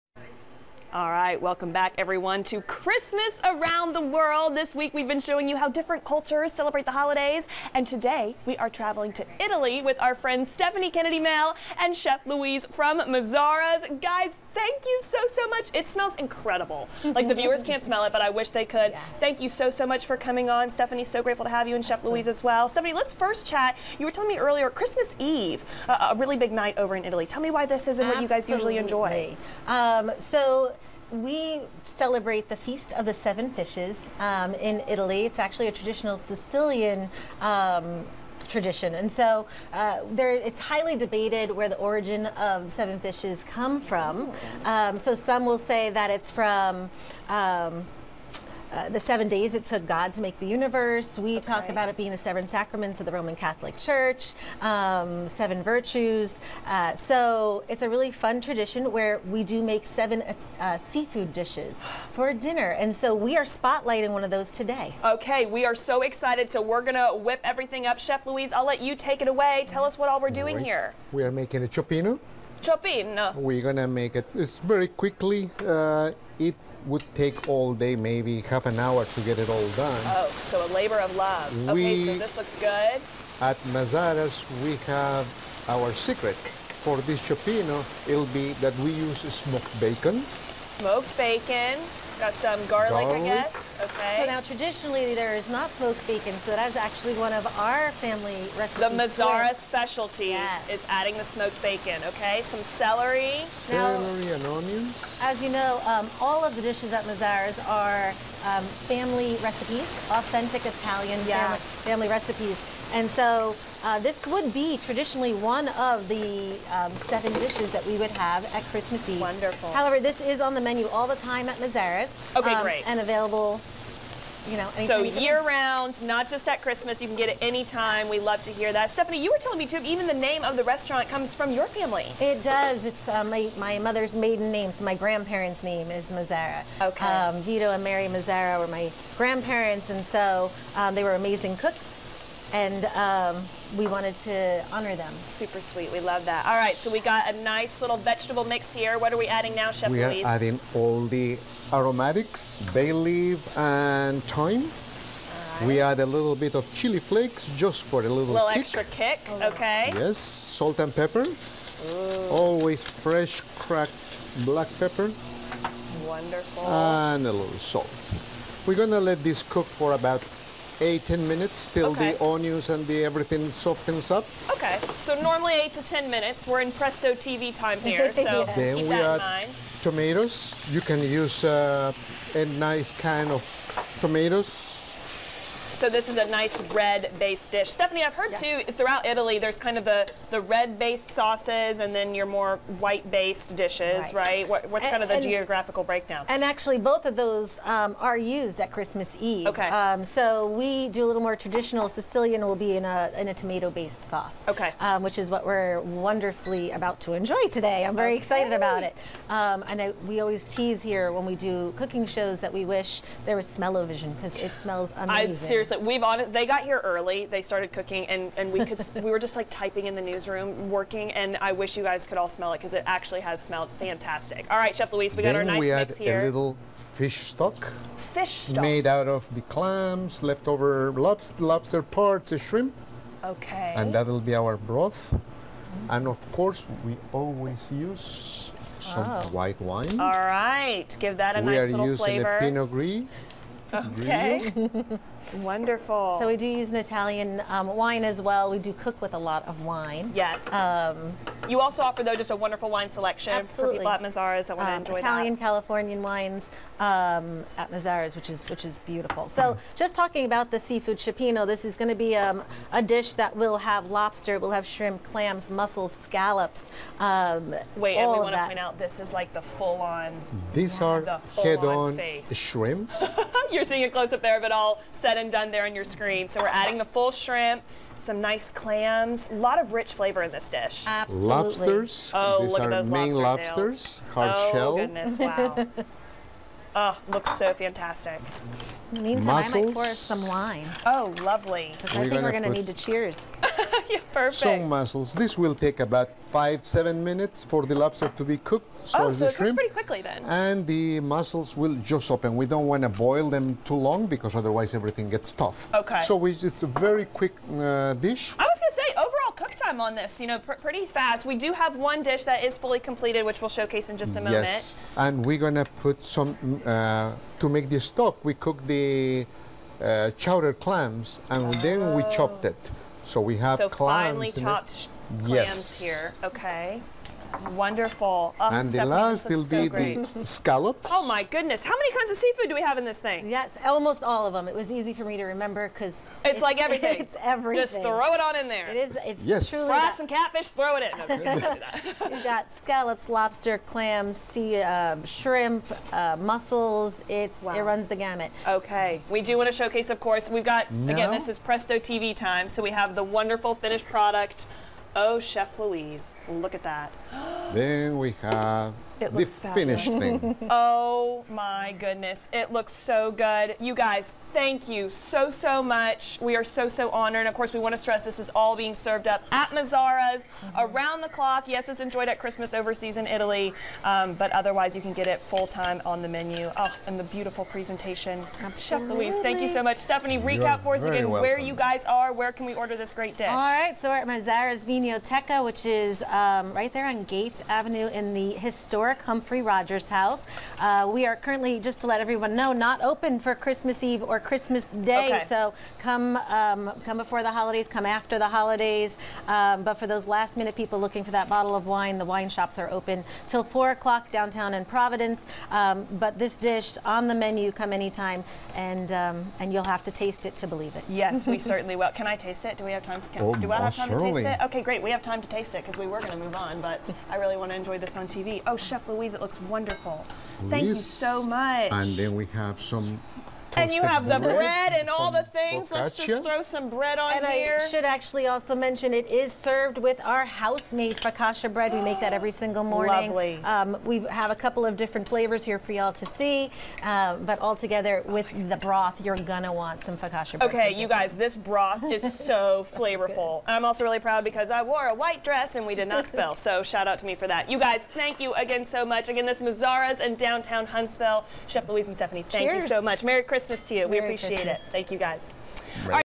Christmas Around the World: Italy!
Learn about Italy's holiday traditions with Mazzara's